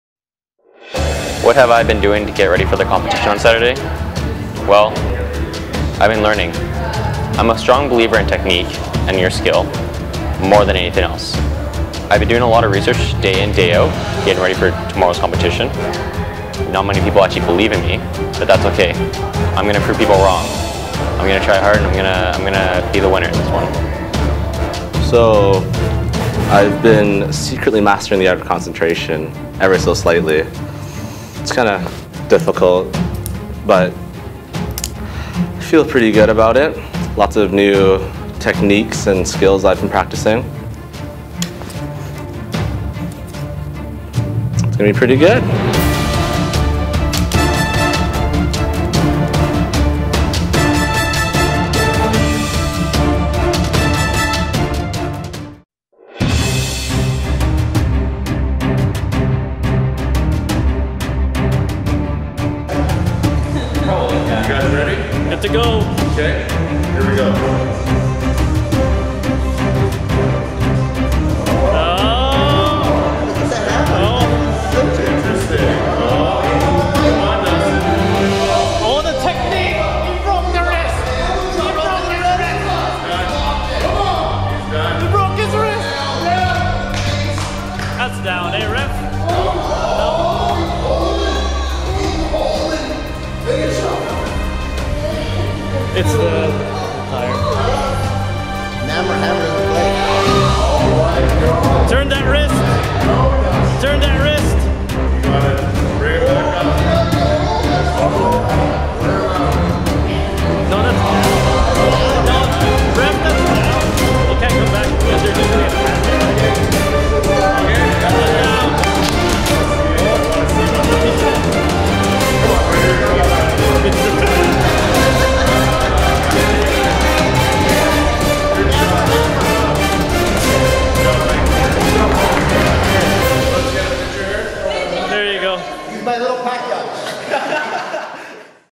Staff Showdown 2 | Mike vs Dustin | Marv Jones Honda
Staff Showdown 2! Dustin returns and challenges Mike to an Arm Wrestling match!